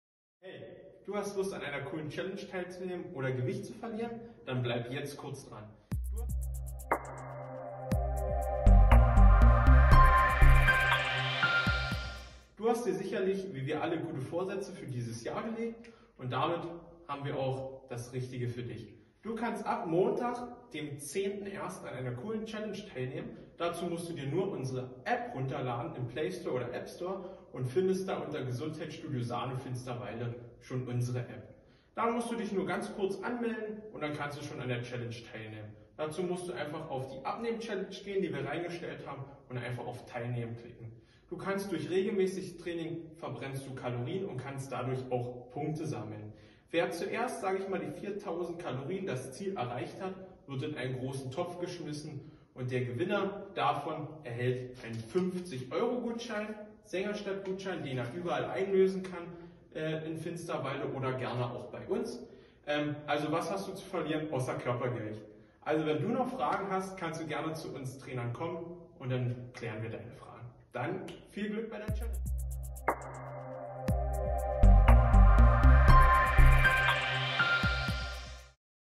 Erfülle ich der Voraussetzungen für die Abnehmchallenge?
⚠️ An die SANO-Familie❤️: Du möchtest im neune Jahr Abnehmen und etwas für deine Gesundheit tun?🤩🏃🏻🏃‍♀️🍀

Mit der  30-Tage Abnehmchallenge wollen wir dich im SANO unterstützen.🤝

Wie läuft das ganze nun ab❓

Gesund natürlich! Hoffentlich. Jeder von Euch kann an dieser Challenge teilnehmen. ☑️

Spielzeitraum: Teilnahme zu Jederzeit möglich🕐🔔
Melde dich in der SANO Finsterwalde App bei der Challenge an (auf den Button "Teilnehmen" drücken).
Bei jeden Trainingsbesuch verbrennst du Kalorien (täglich können bis zu 400 Kalorien verbrannt werden)
Im Spielzeitraum vom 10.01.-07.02. können verbrannte Kalorien gesammelt werden.
Hast du es bis zum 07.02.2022 geschafft, das Ziel von 4000 Kalorien zu erreichen, wirst du in den Lostopf geworfen.🗳
Am Ende des Spielzeitraumes wird der Gewinner des 50-EURO-Sängerstadtgutscheines ausgelost.💶
Möge der Zielstrebigste gewinnen! ;-)